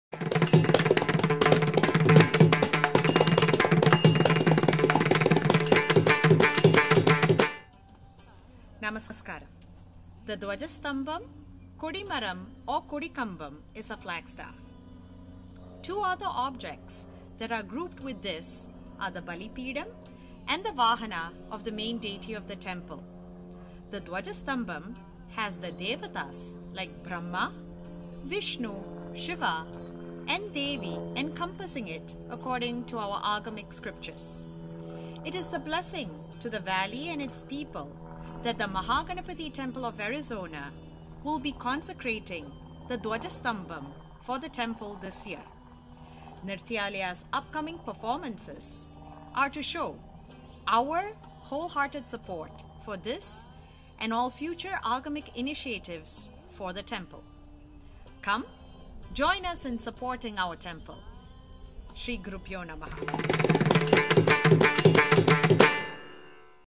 Agamic Beats - Nrityalaya Aesthetics Society
Nrityalaya Aesthetics Society brings a vibrant and fast paced program on March 1 and March 8.